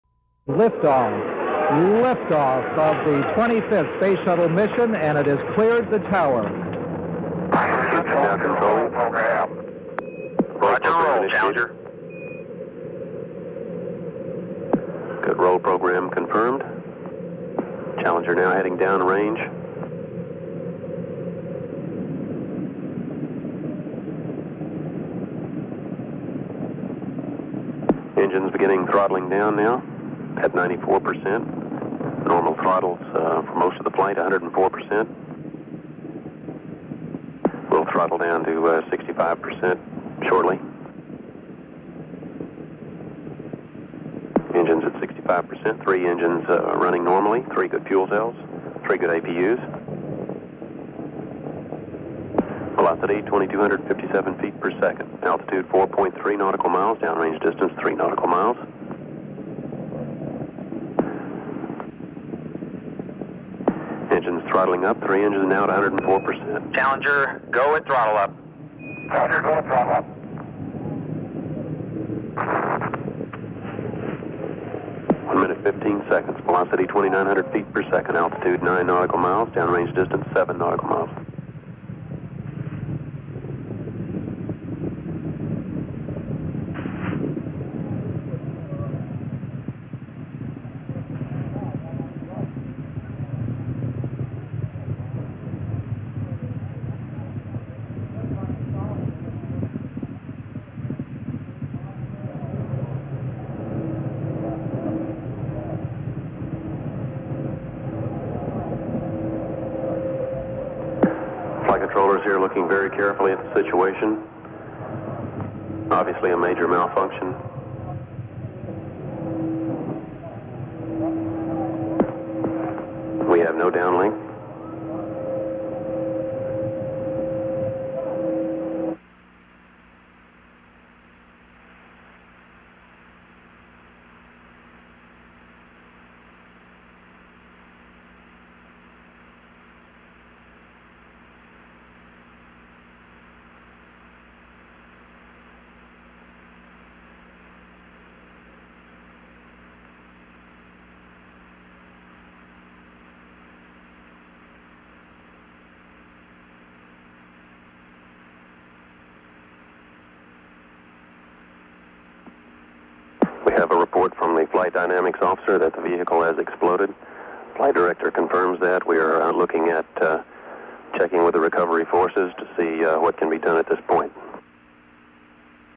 Space shuttle Challenger exploding during launch
(28 Jan 1986) Space shuttle Challenger exploding during launch